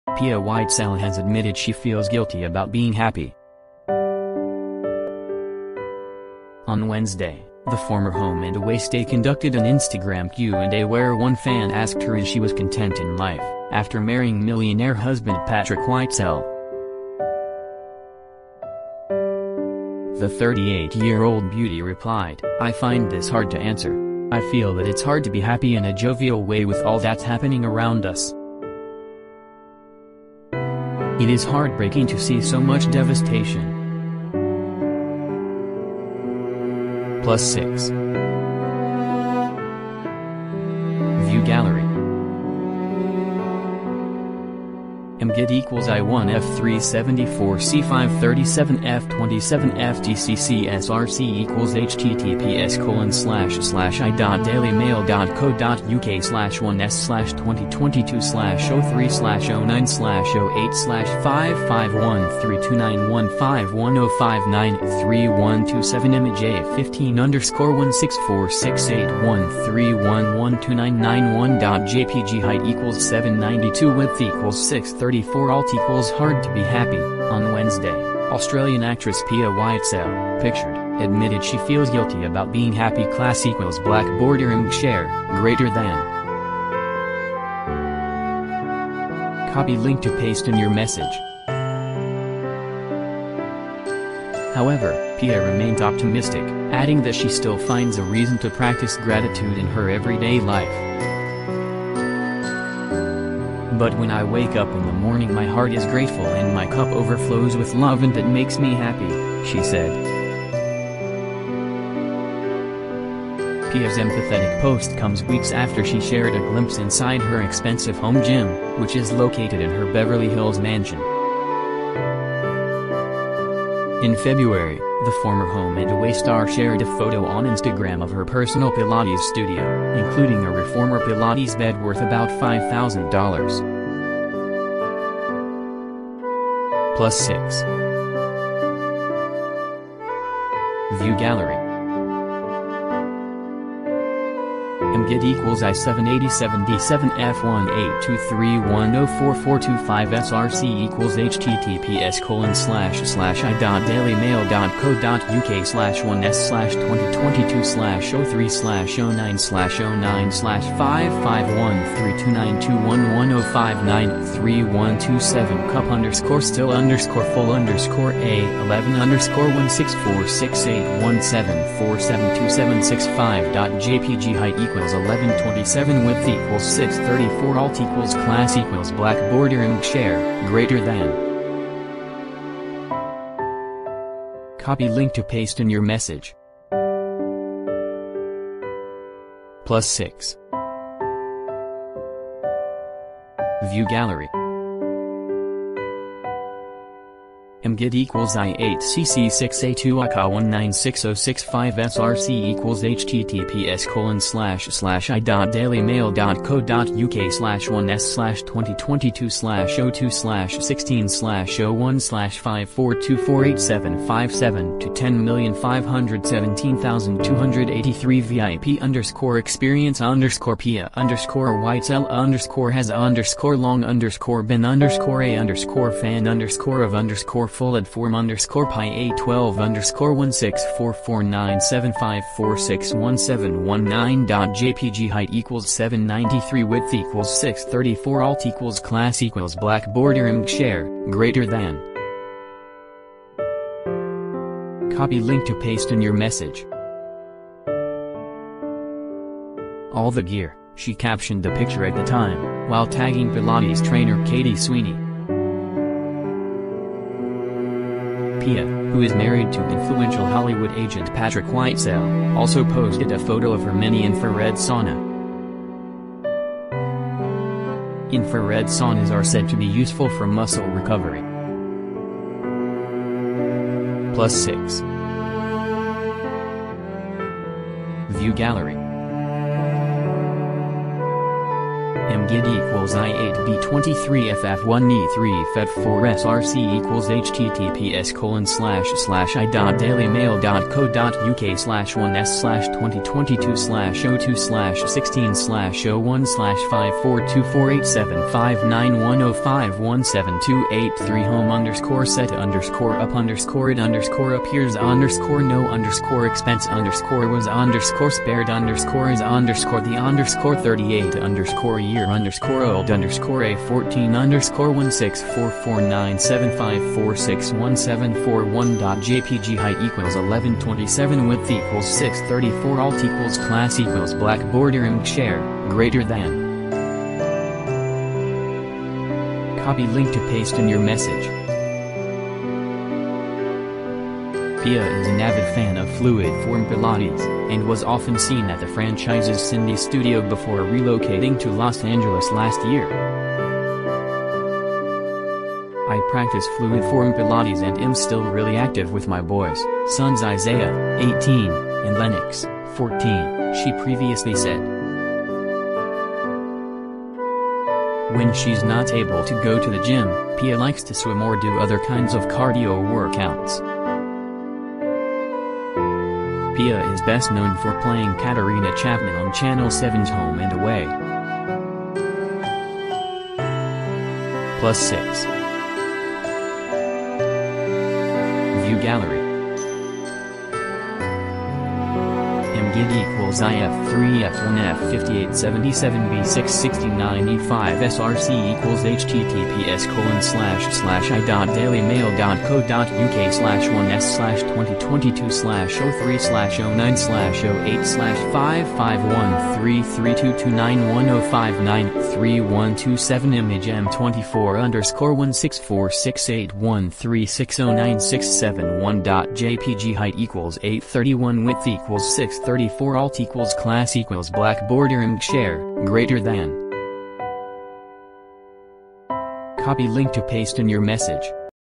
Pia Whitesell admits she feels guilty about being 'happy' during 'devastating' time in the world
Pia Whitesell has admitted she feels guilty about being 'happy'.
On Wednesday, the former Home and Away stay conducted an Instagram Q&A where one fan asked her is she was content in life, after marrying millionaire husband Patrick Whitesell.
The 38-year-old beauty replied: 'I find this hard to answer. I feel that it's hard to be "happy" in a jovial way with all that's happening around us.
'It is heartbreaking to see so much devastation.'
+6
View gallery
img id="i-1f374c537f27fdcc" src